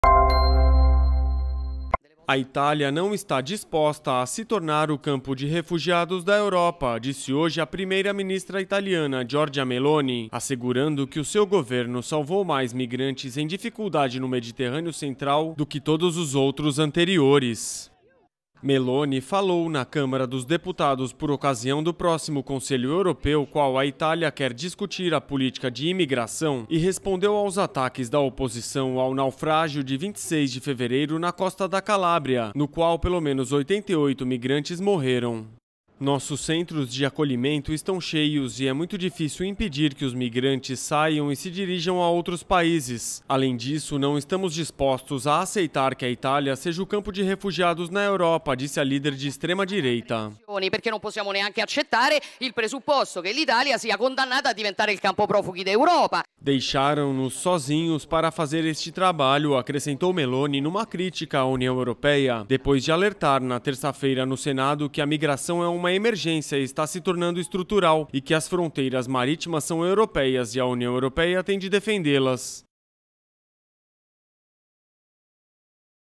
Meloni diz que Itália não está disposta a "se tornar o campo de refugiados da Europa"
Roma, 22 mar (EFE).- A Itália não está disposta a "se tornar o campo de refugiados da Europa", disse hoje a primeira-ministra italiana, Giorgia Meloni, assegurando que o seu governo salvou mais migrantes em dificuldade no Mediterrâneo Central do que todos os anteriores.